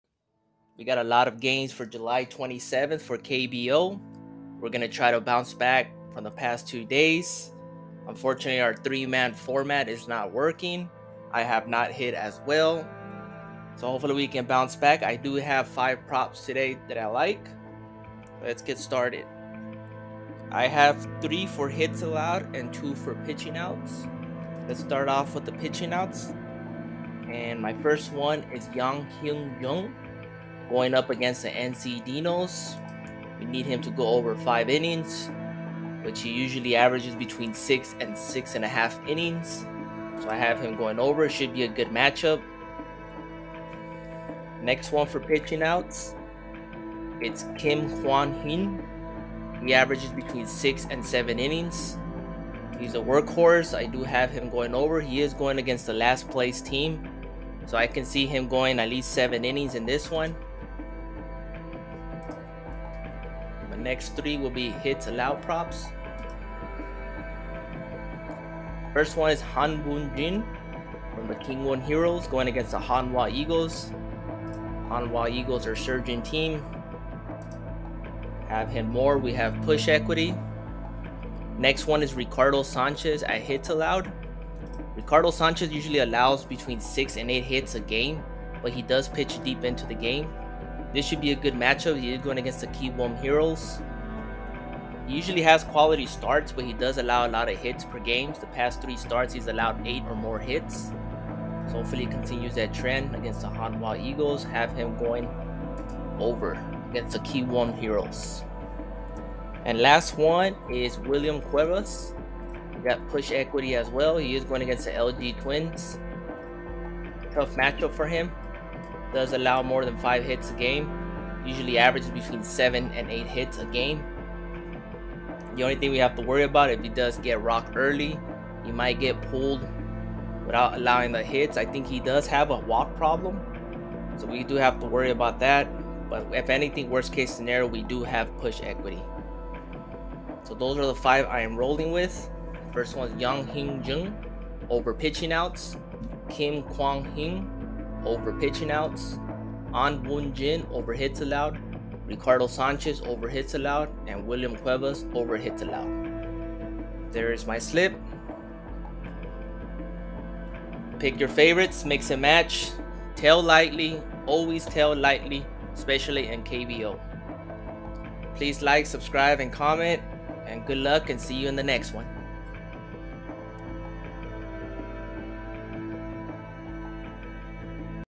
Prize Picks KBO July 27th Props
Prize Picks KBO Picks for July 27th. Tail lightly
Tail at your own risk or pick the one you think it will hit